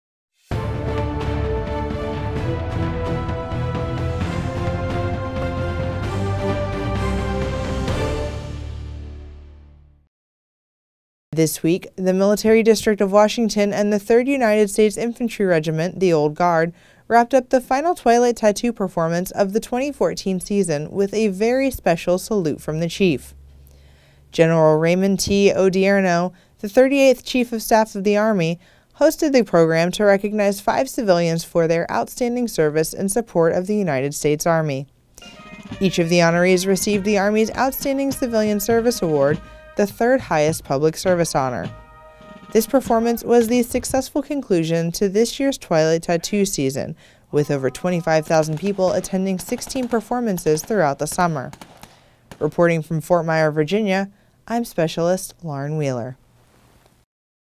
Salute From The Chief
The Military District of Washington and soldiers from the 3rd United States Infantry Regiment, The Old Guard, performed their final Twilight Tattoo of the 2014 season. Hosted by General Raymond T. Odierno (38th Chief of Staff of the Army), this special Salute from the Chief performance honored 5 civilians who exemplify service and commitment to the military. Each was awarded with the Outstanding Civilian Service Award during the ceremony. (Army video by SPC Lauren Wheeler)